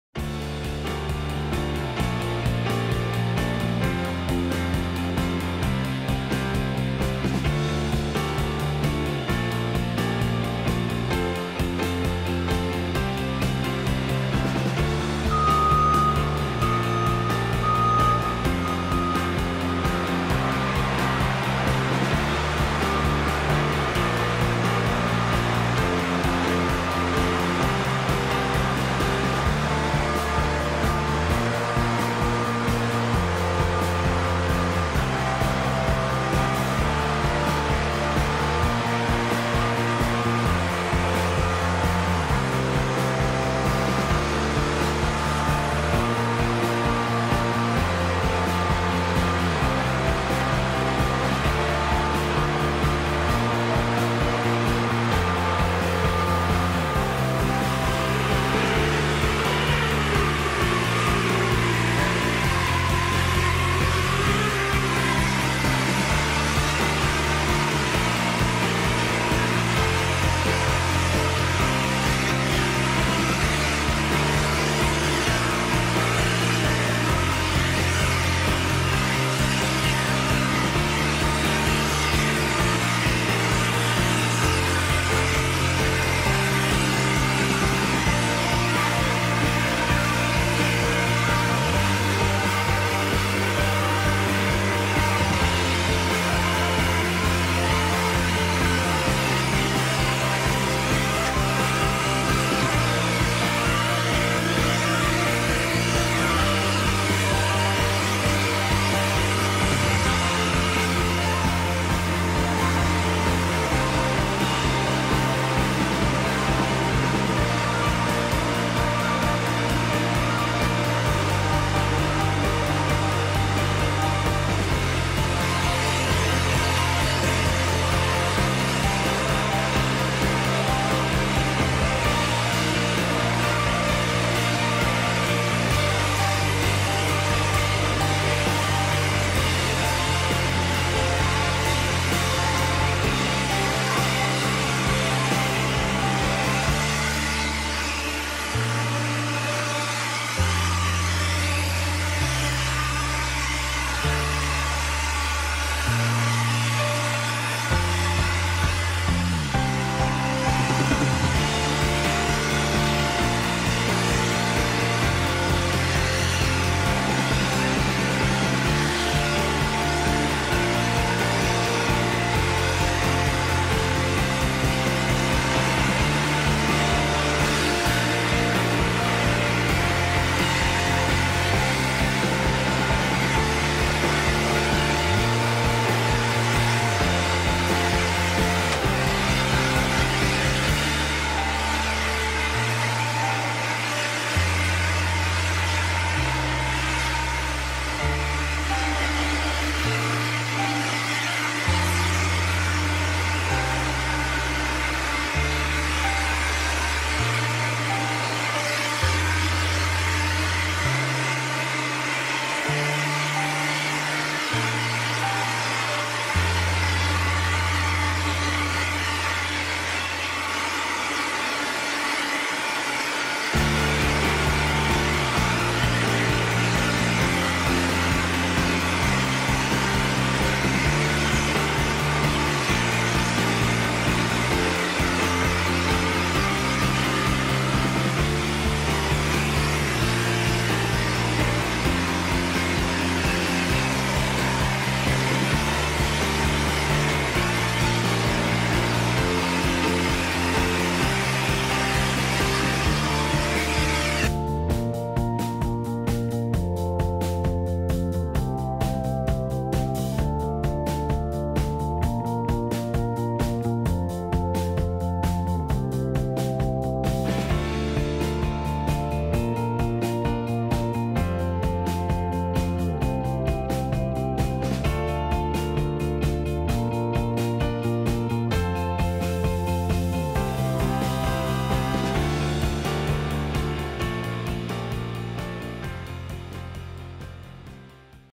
Incredible In Your Face HD Video of Fire Proofing with Grace Monokote Fireproofing
Watch these amazing close up video shots where our video team gets so close to Penta's spray guy that fire cement is literally landing on our equipment. It's worth the sacrifice though, because you get to see HD footage (shot in 4K and downscaled ) probably never recorded before, of fire proofing taking place on a mezzanine steel structure. Here you will see John, a chap with 27 years experience in fireproofing, spray a Grace Monokote MK6 fire cement onto the underside of the steel beams in this depot building to give the structure a 1 hour fire rating . The 'fire mud' is pumped by a Putzmeister S5EV fireproofing machine to the nozzle where the mud is atomized and blasted against the steel structure. Penta Protective Coatings specialize in fireproofing, fire-stopping , and spray-foam .

The video was shot with a Samsung NX1 on a 12mm-24mm Samsung lens using a Nebula 4000 Steadicam . The footage was recorded on full auto set at maximum 4k resolution in H265  at 24 frames per second and then downscaled to H264 24 frames